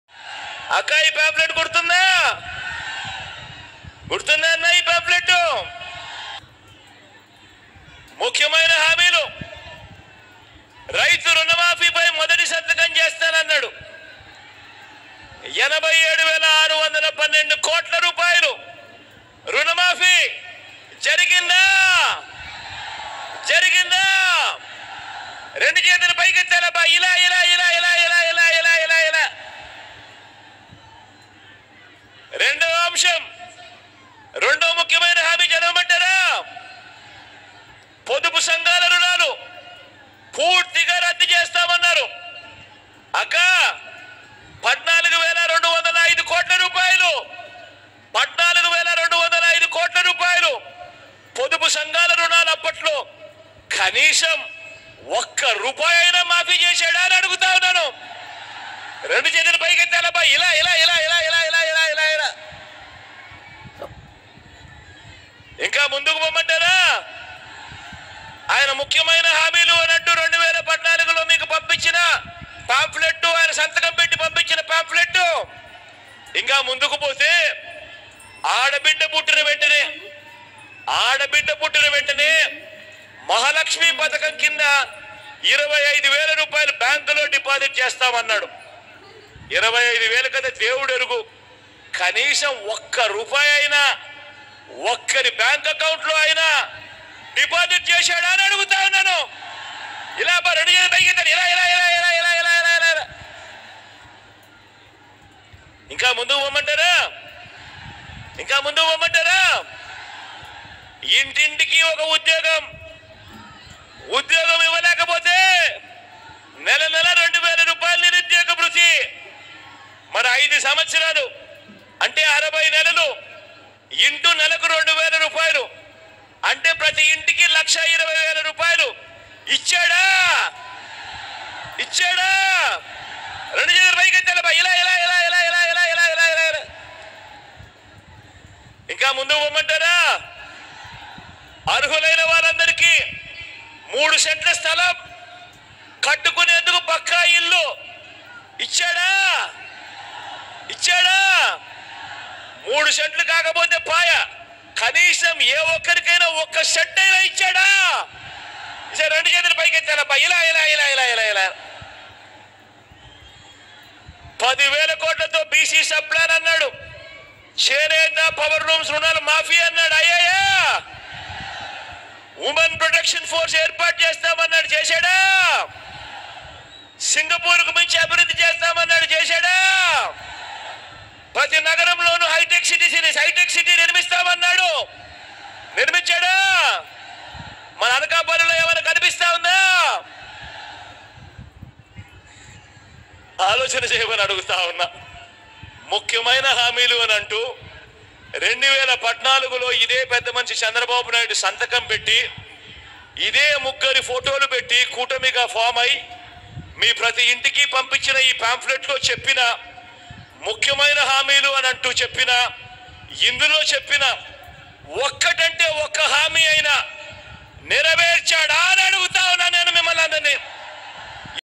అనకాపల్లి సిద్ధం సభలో రుణమాఫీ ప్రకటించిన జగన్ ap cm Jagan siddham meeting on rythu Runa maafi.
hello everyone today update is AP CM Jagan meeting on rythu Runa maafi.@anandsocialedu